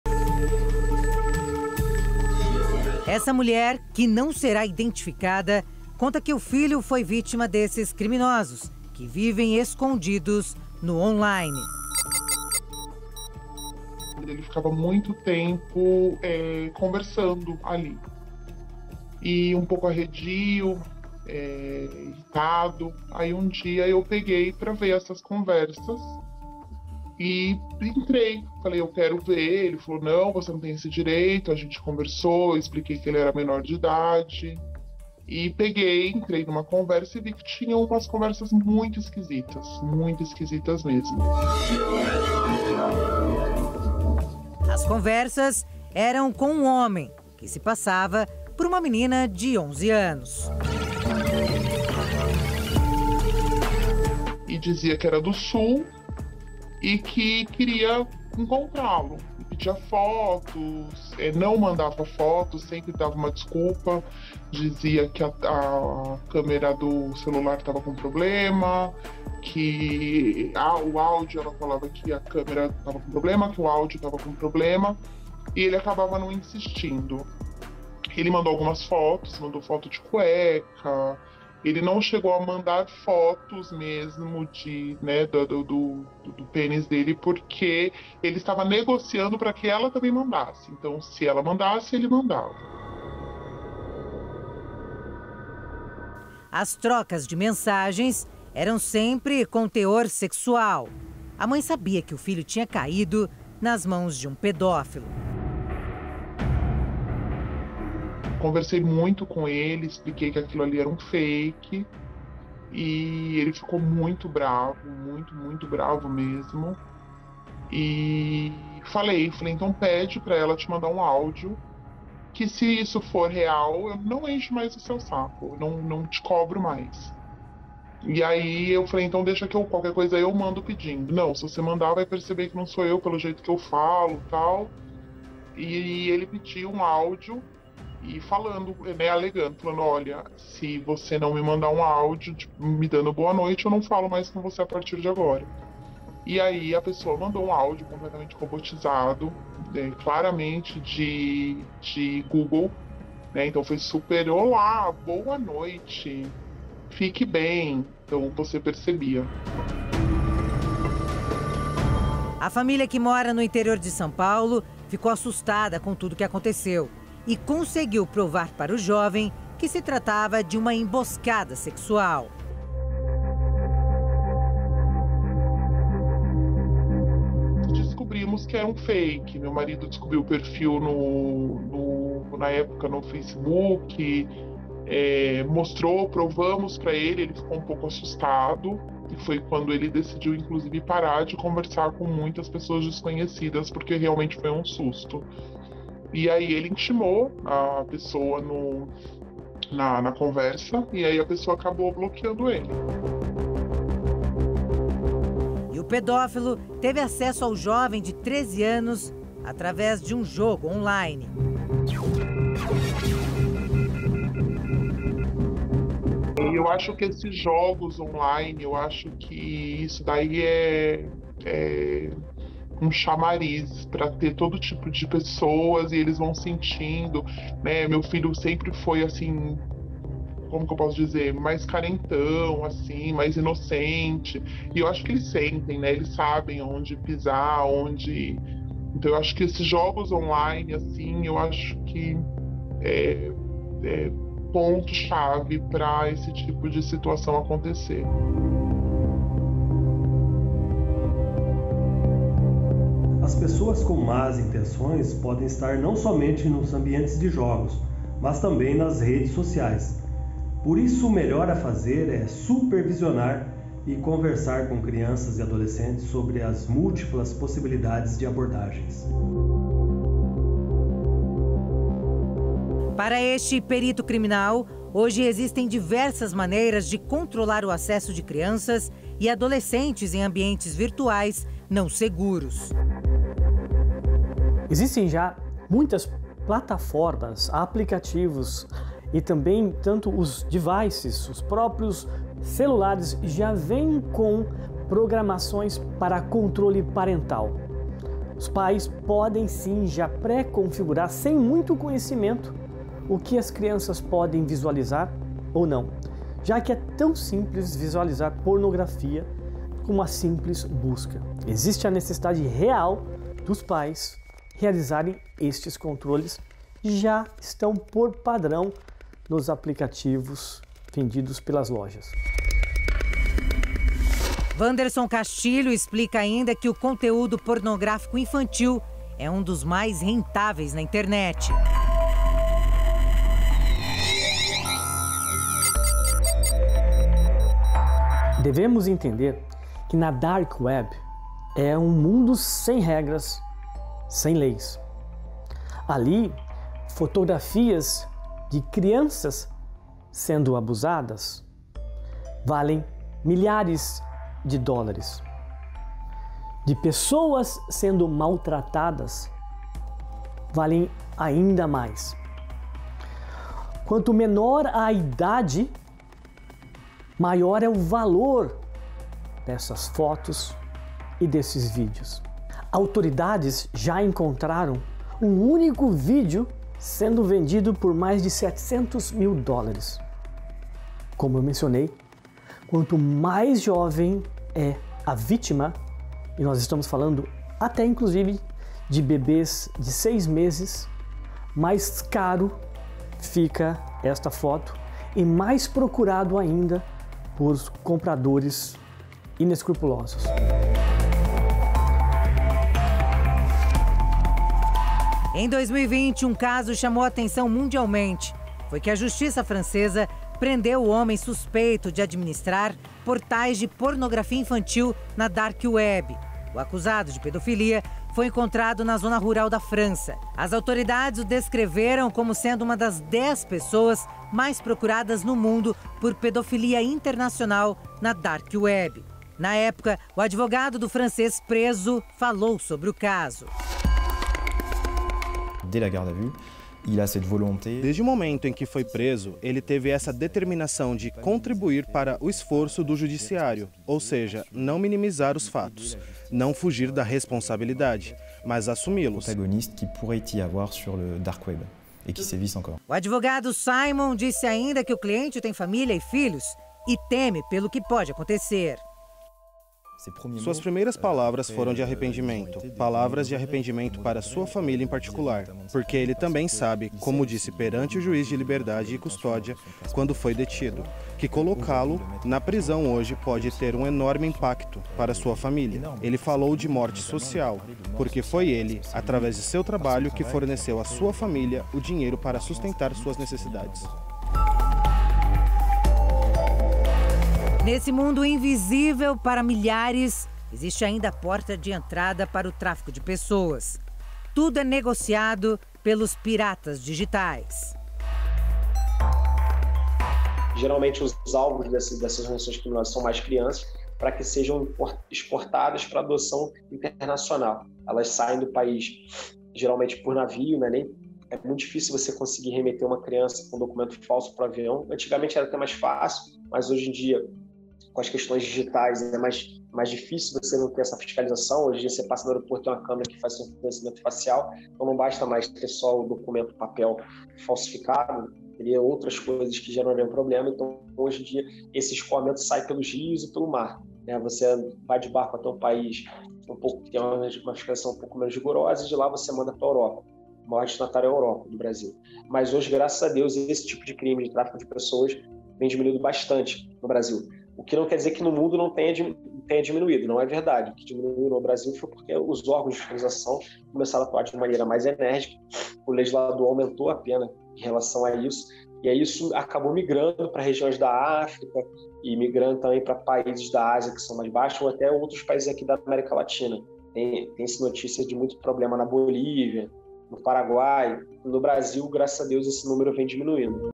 Como proteger crianças e adolescentes das 'camadas obscuras' da internet | DOCUMENTO JP
O Documento Jovem Pan traz o relato de uma mãe que afirma ter percebido a mudança nas atitudes de seu filho, menor de idade.